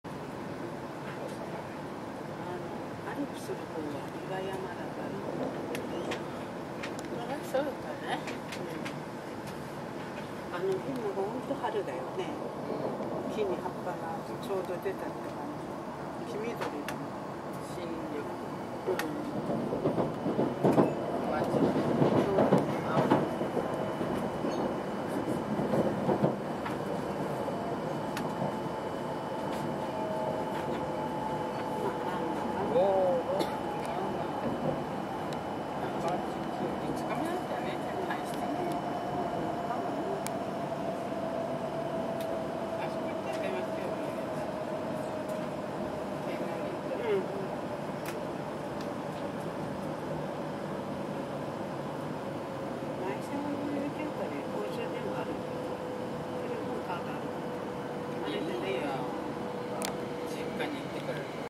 Eine Reise mit dem Shinkansen im Mai nach Nord Japan Iwate
Shinkansen die schnellsten und pünktlichsen Züge der Welt